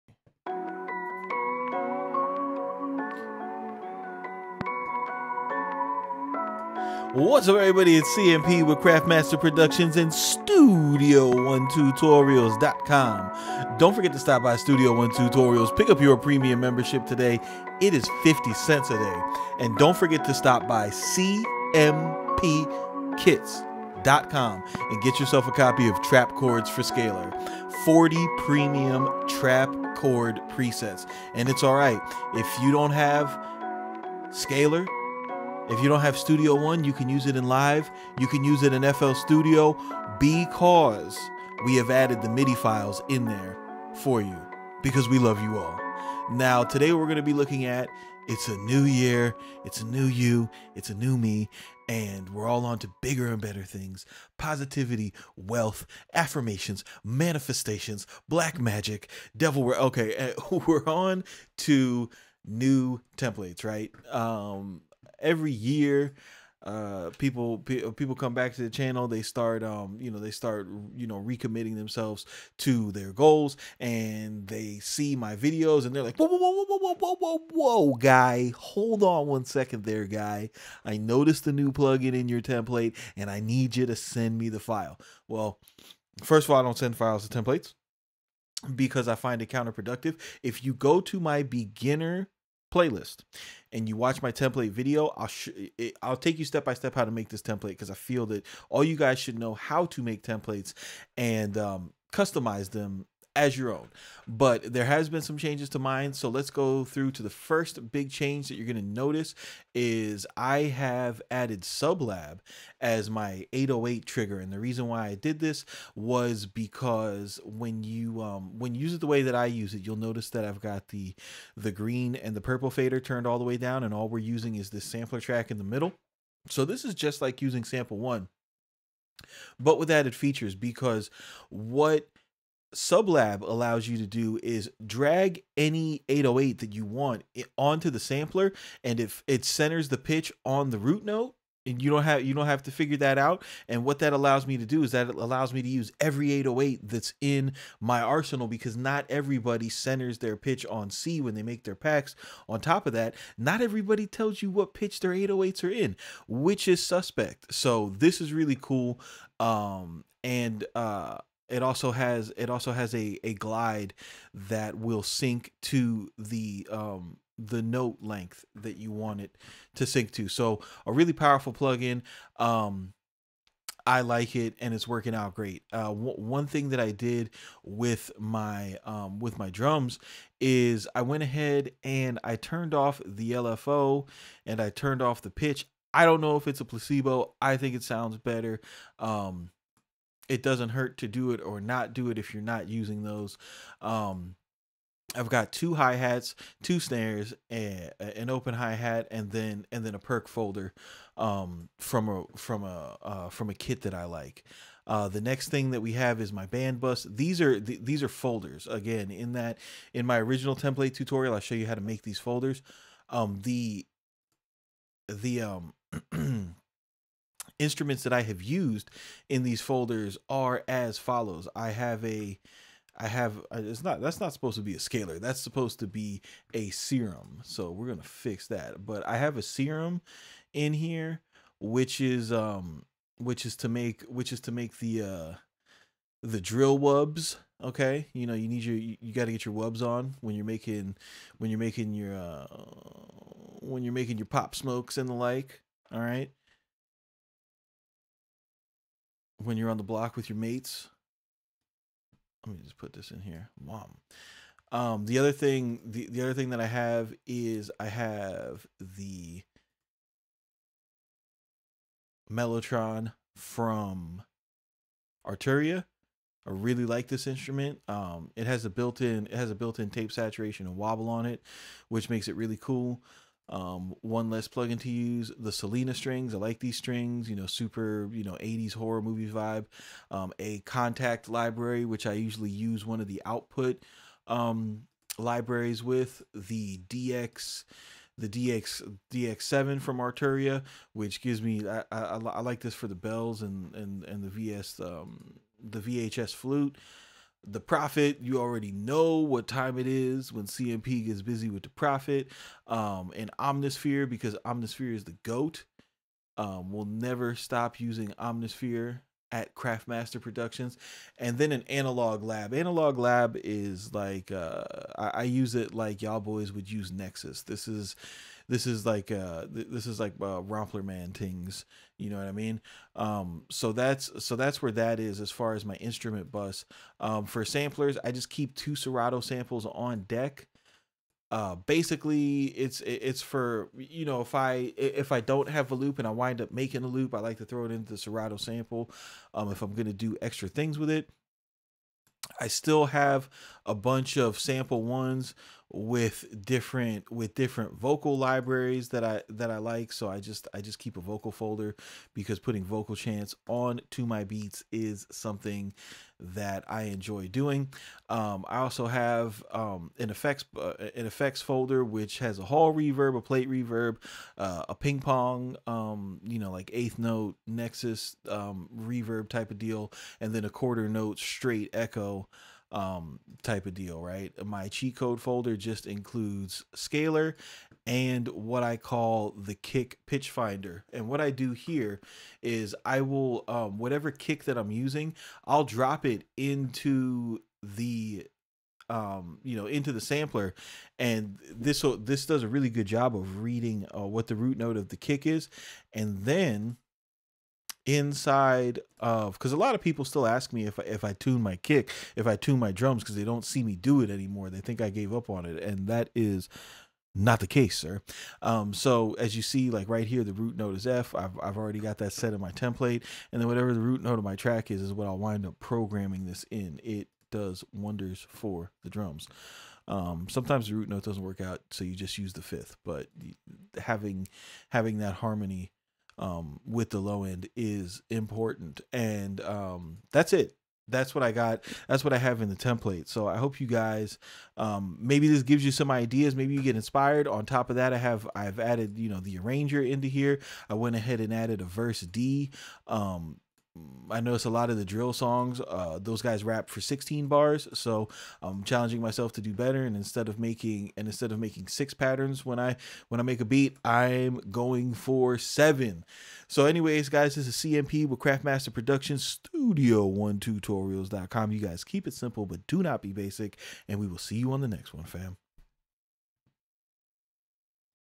My New DRILL Template for Studio One 2020
In this edition we take a look at My New DRILL Template for Studio One 2020